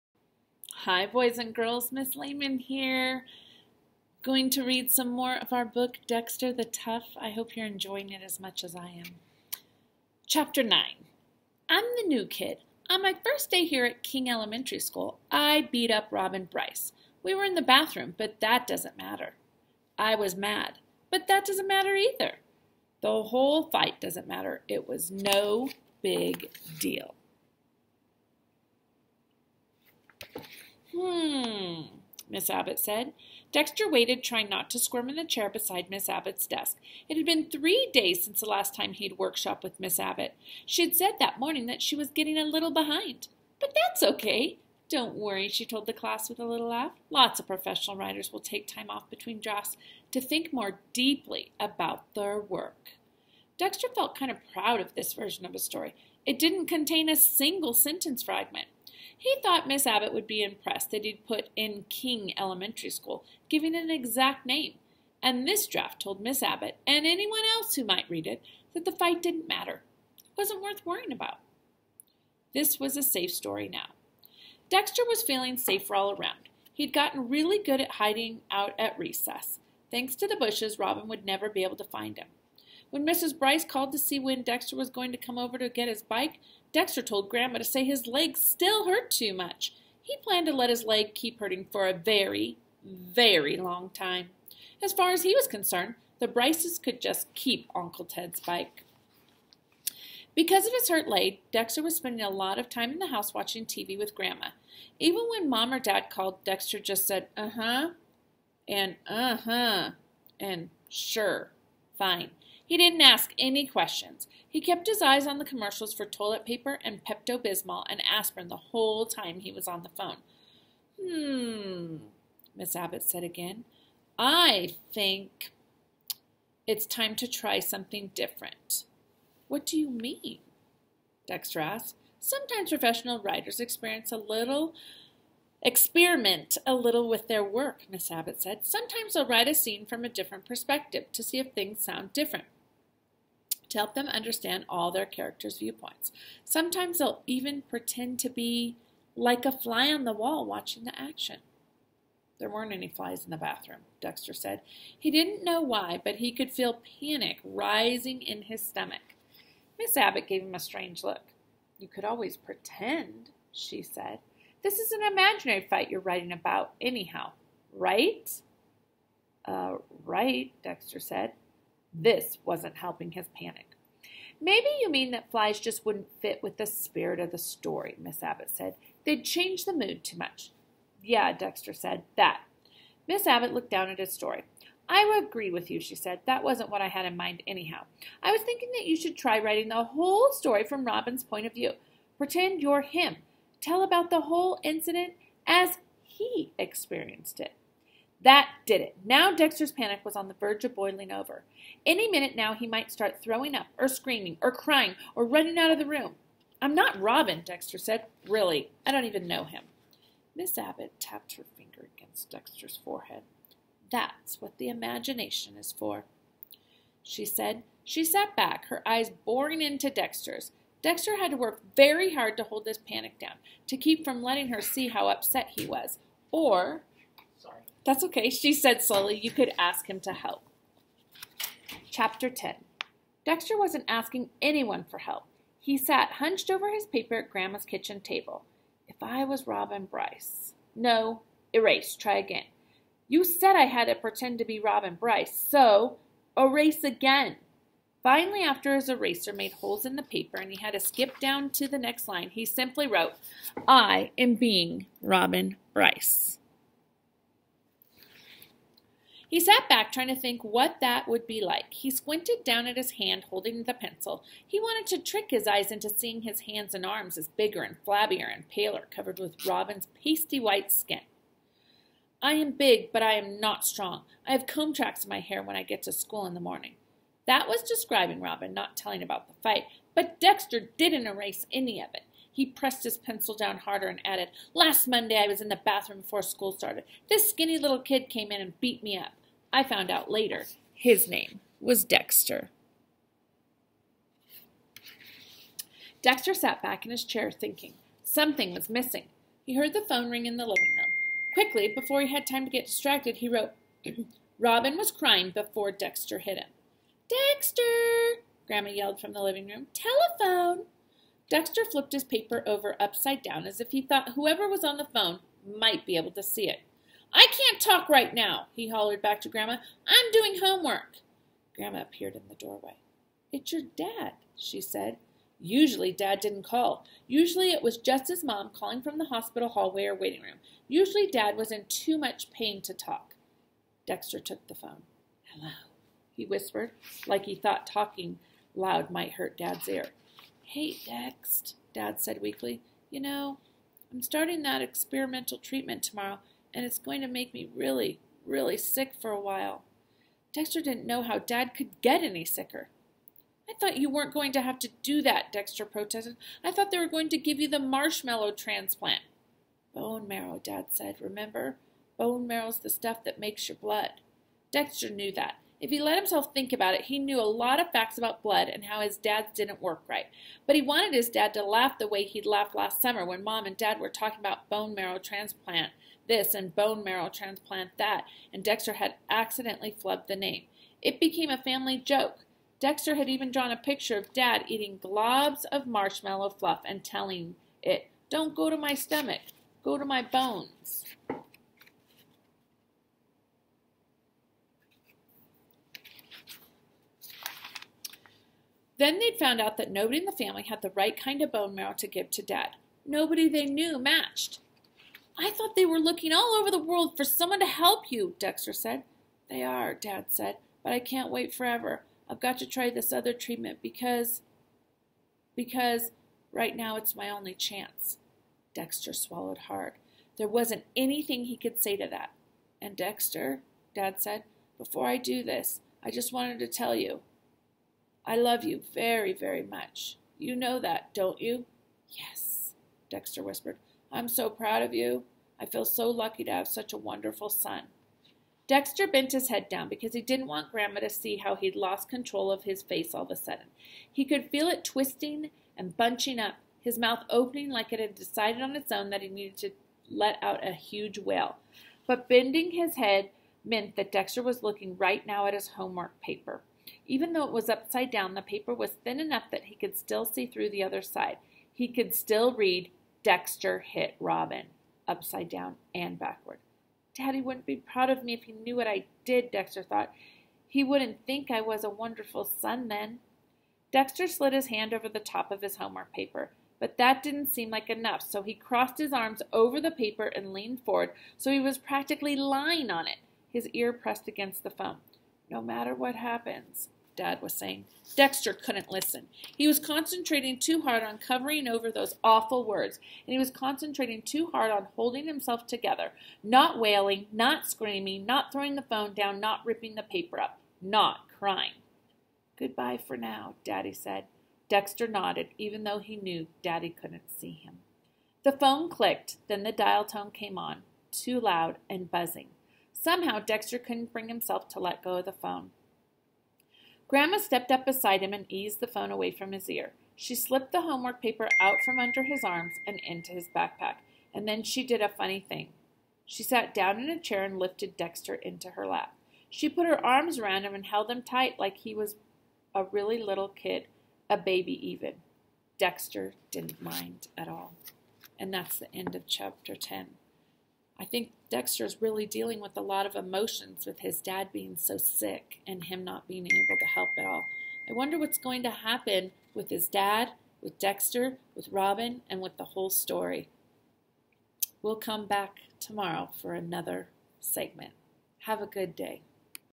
Dexter the Tough Chapters 9 and 10
Ms. Lehmann reads chapters 9 and 10 of Dexter the Tough